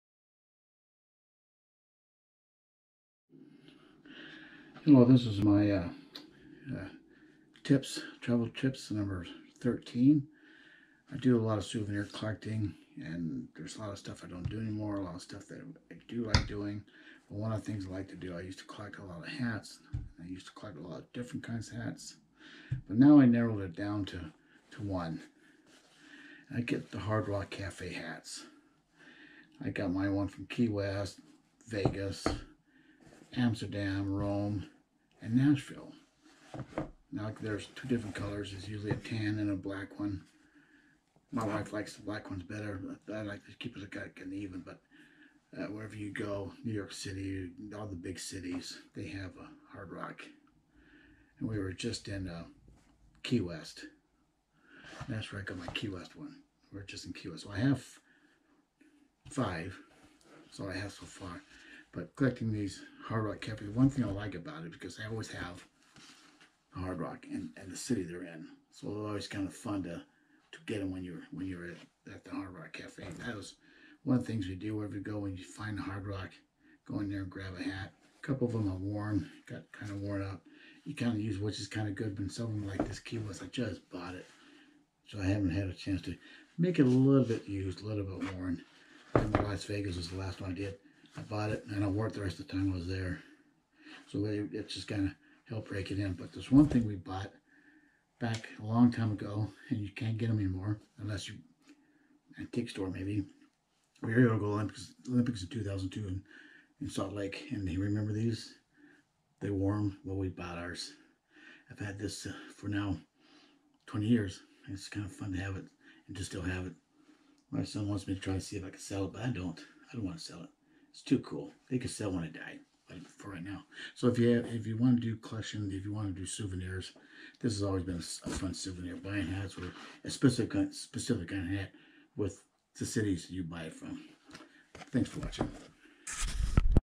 Travel Tips #13, Hard Rock Cafe Hats
The Hard Rock Cafe hats have the city's name on them.  So, whenever I'm in town, I look for a Hard Rock & buy a hat!